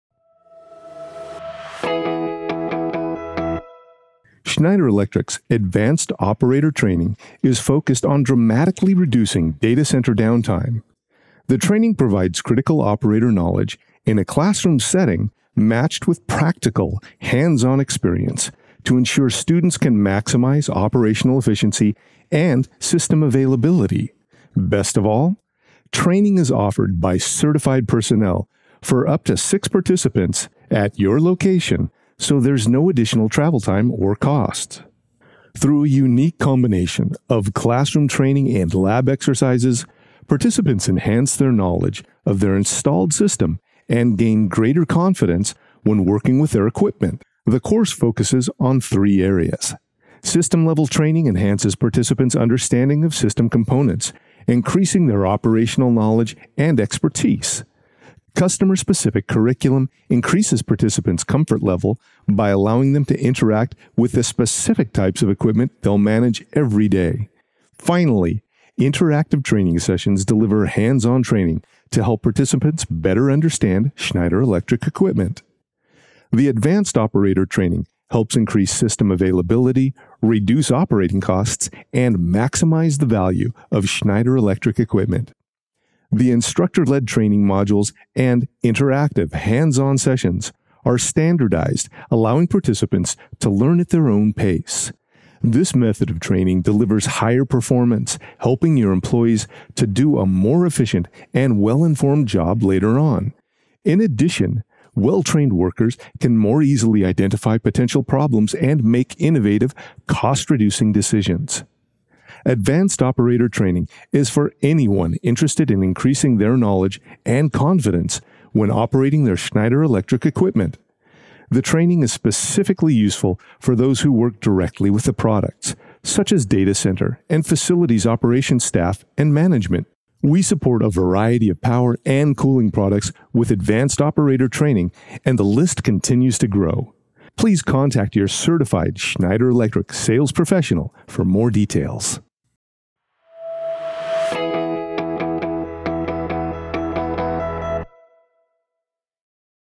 Advanced Operator Training Services by Schneider Electric
The Schneider Electric Advanced Operator Training service provides the critical operator knowledge and practical experience needed to maximize operational efficiency and system availability, enabling customers to receive the best return on investment from their Schneider Electric system.